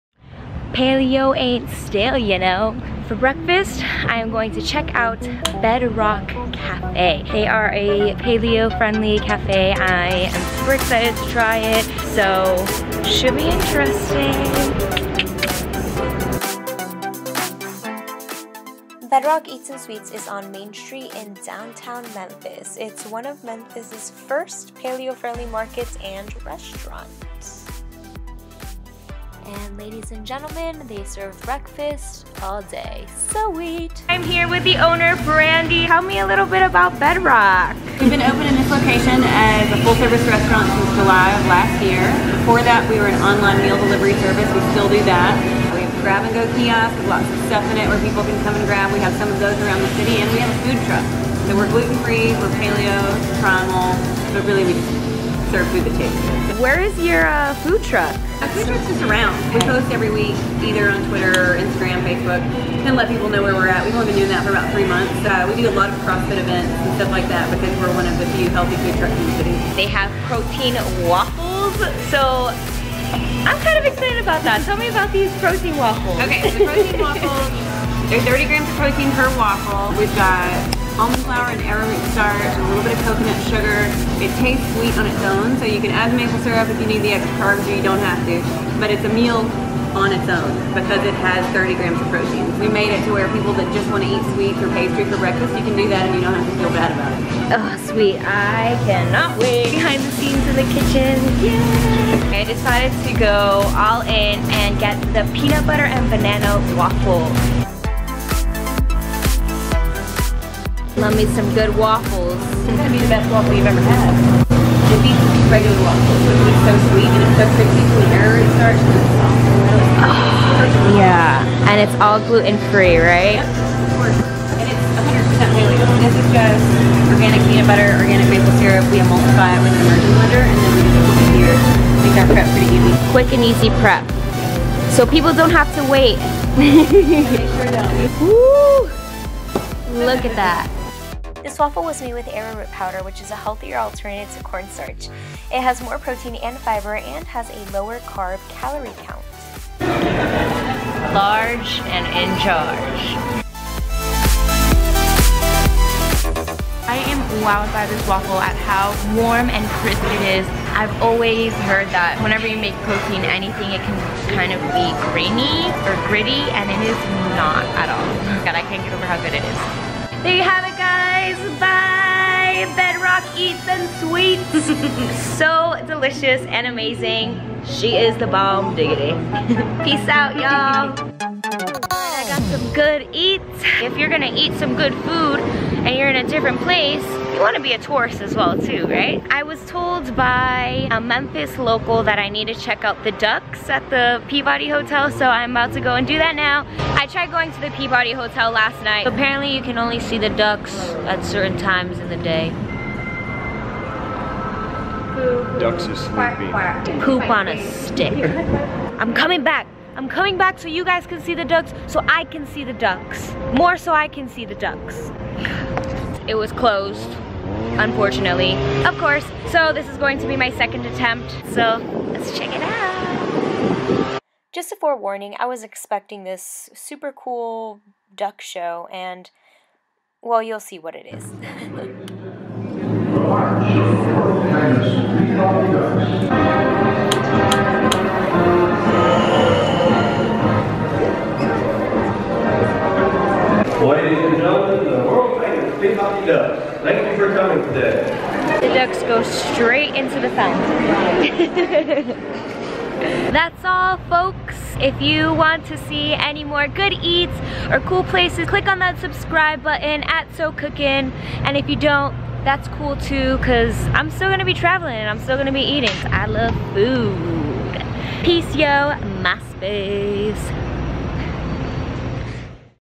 Paleo Protein Waffles! Memphis
Bedrock Eats and Sweets in downtown Memphis is AMAZING. Nuff said. Brandi's food is to die for, she keeps it real and is succhhh a personable woman.

I got a paleo pecan pie to go and wanted to show you guys, but I ended giving it to a homeless man :(. If I'm ever back in Memphis I will most definitely come back and try that pie!

Btw they were waffles not pancakes. Hah. Whoopsieee I made a poopsie 🙈😹